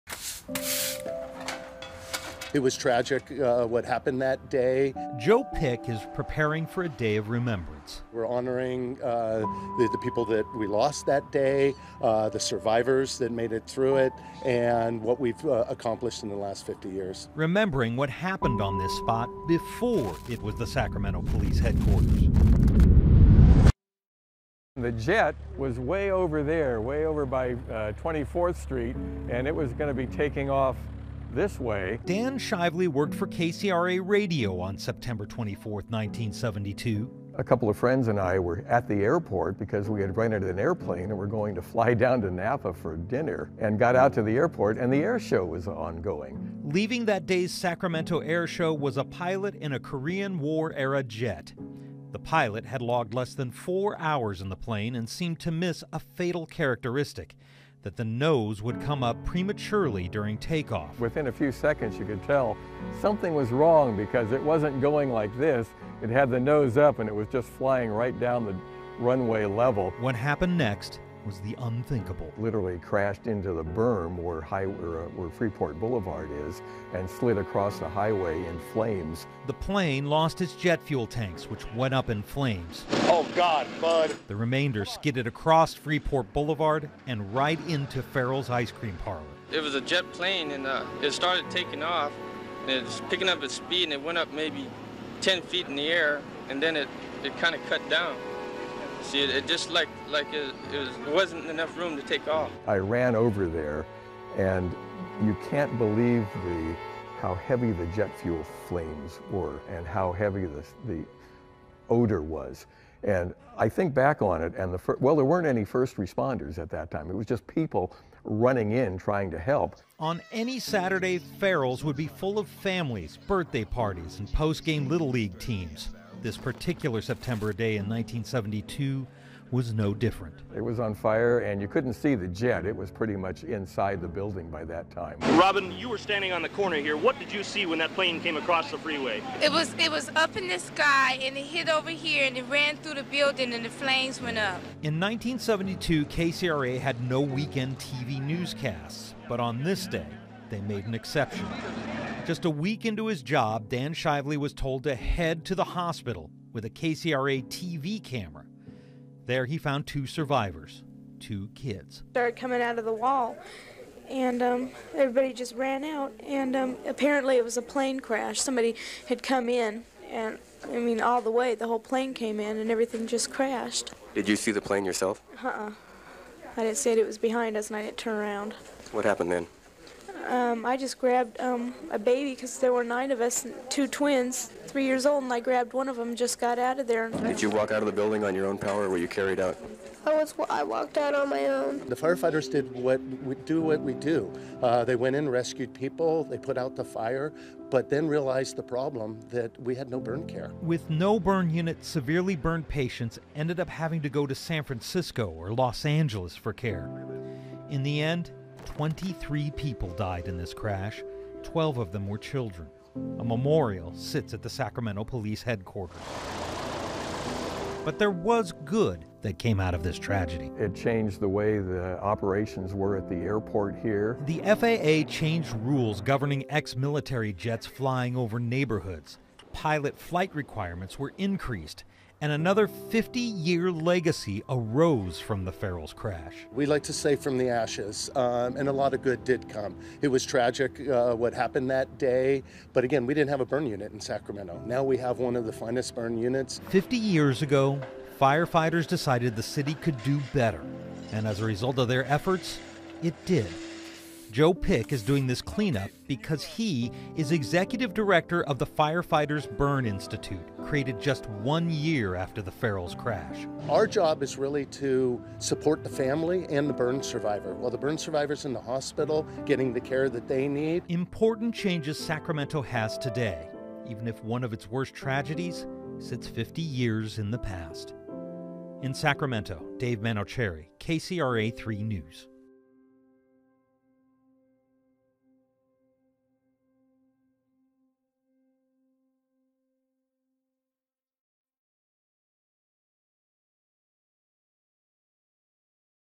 These are lessons learned 50 years after air disaster at Farrell's Ice Cream Parlor in Sacramento
What happened on Sept. 24, 1972, in Sacramento is still considered one of the worst air-to-ground disasters in history.